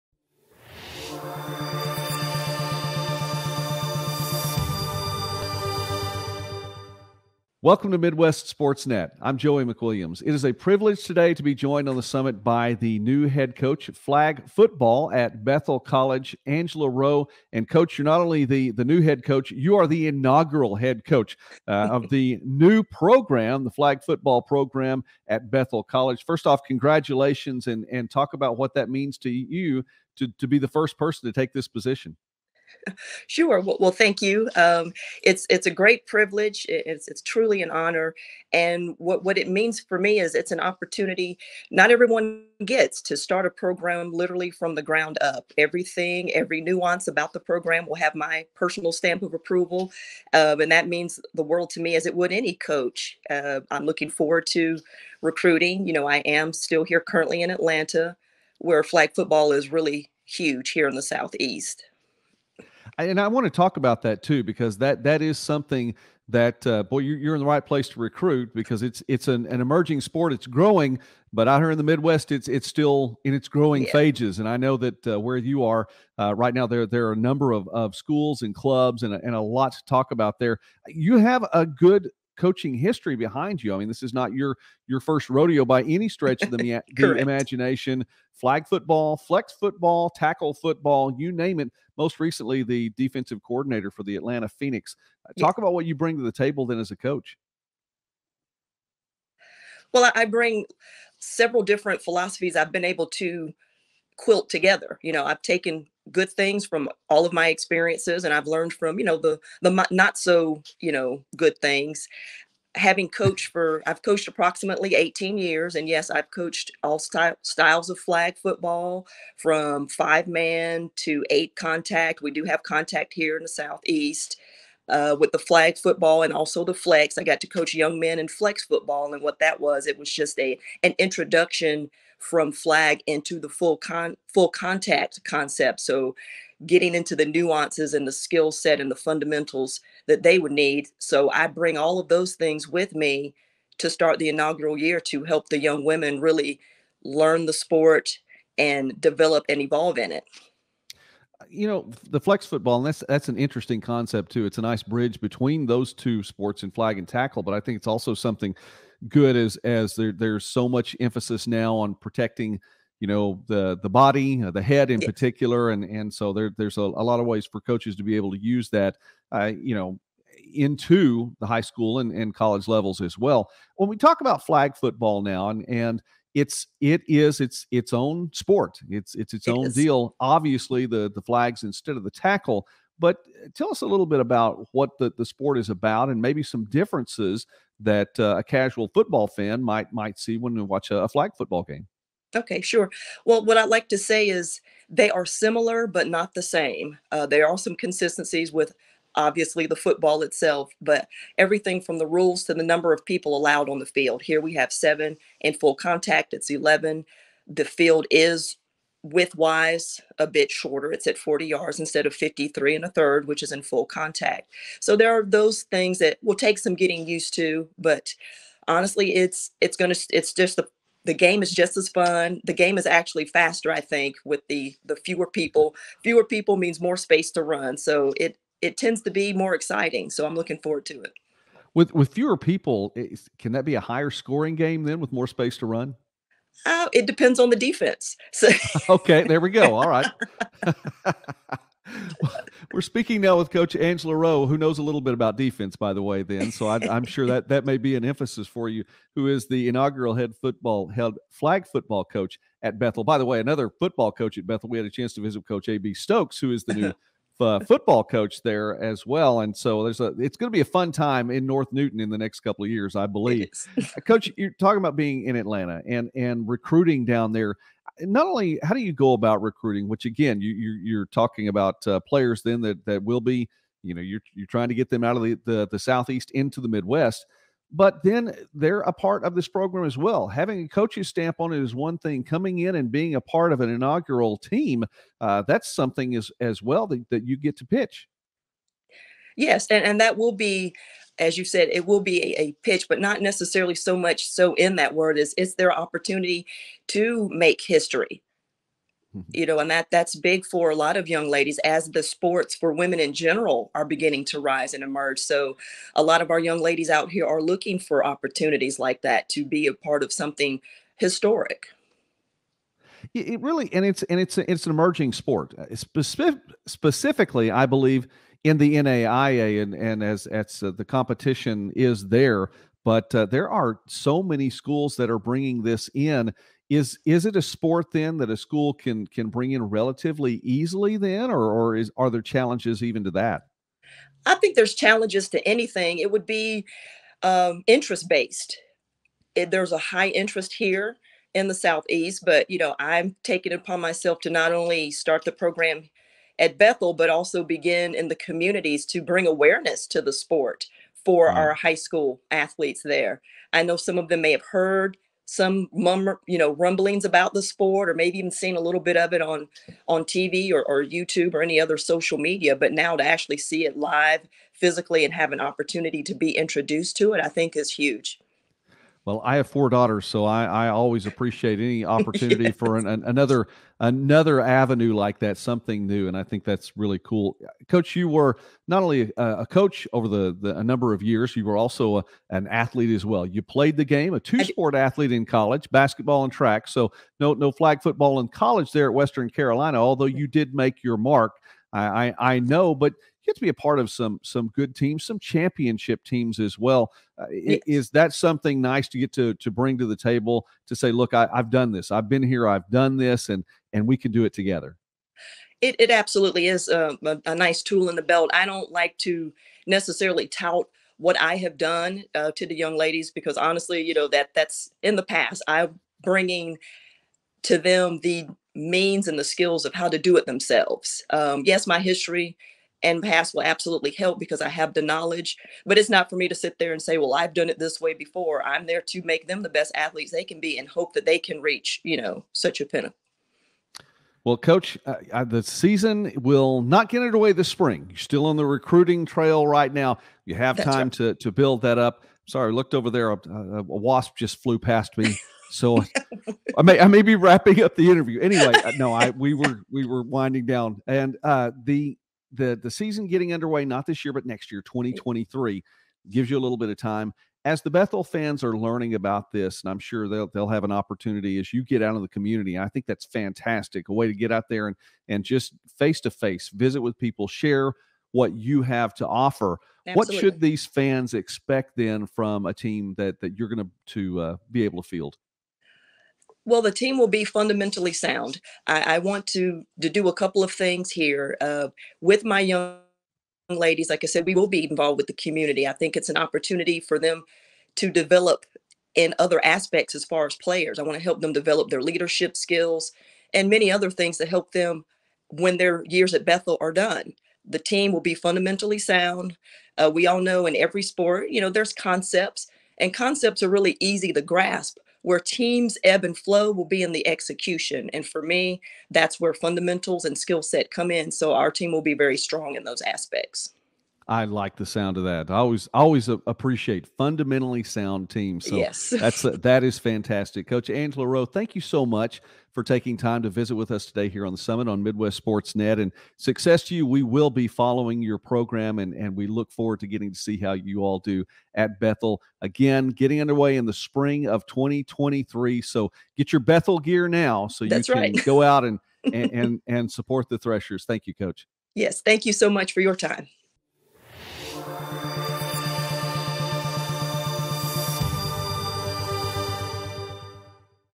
Angela Rowe gets to put her stamp on the new Bethel Flag Football program
The Summit | Angela Rowe, Bethel | No. 124

New coach Angela Rowe gets to put her stamp on the Bethel Flag Football program as the Threshers' inaugural flag coach.